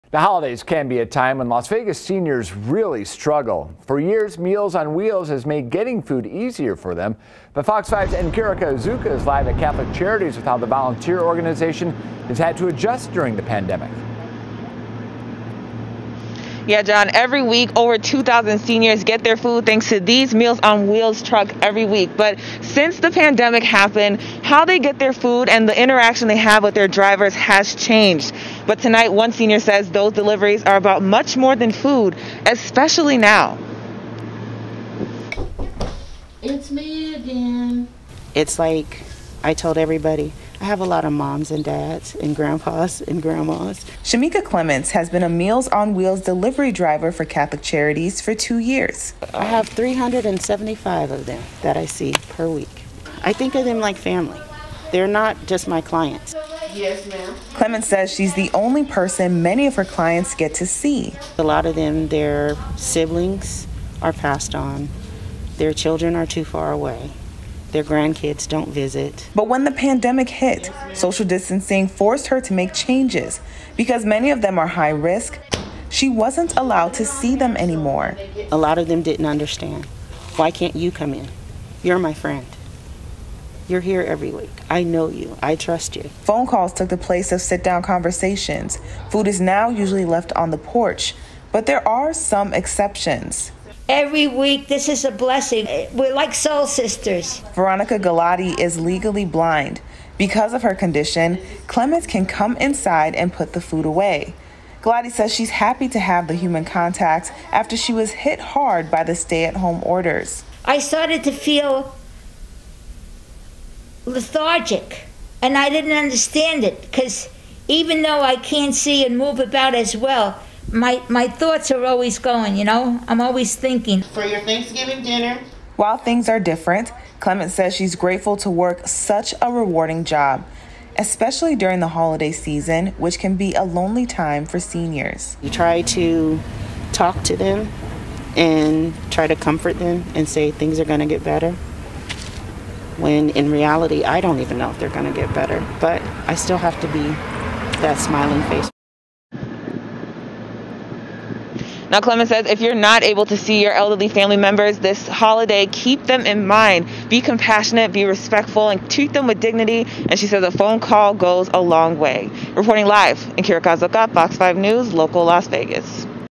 Meal on Wheels staff adjusts to COVID-19 pandemic in order to serves seniors in need
In Las Vegas, Meals on Wheels is helping more than 2,000 senior citizens with food delivery. During the pandemic, the organization has had to adjust their protocol.